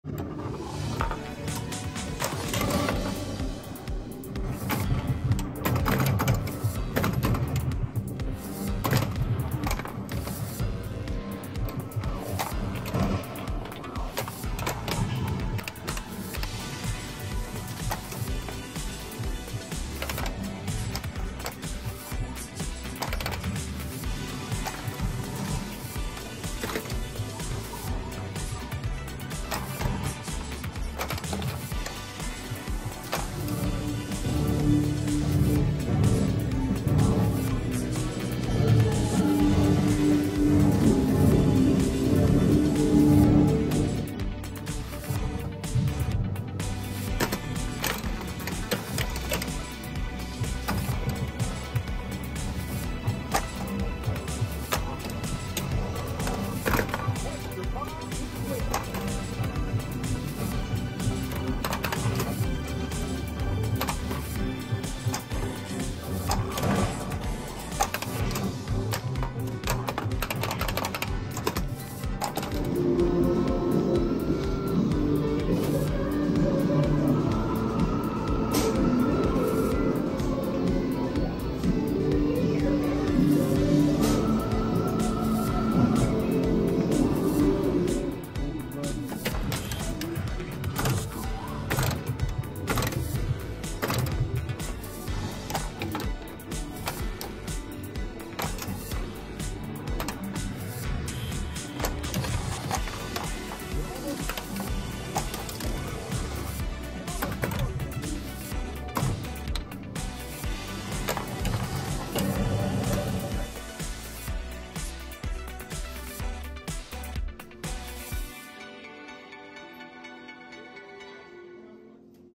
Lit Kit Flippers - Walking Dead Pinball Mod
New wireless projection flippers for Walking Dead pinball machine.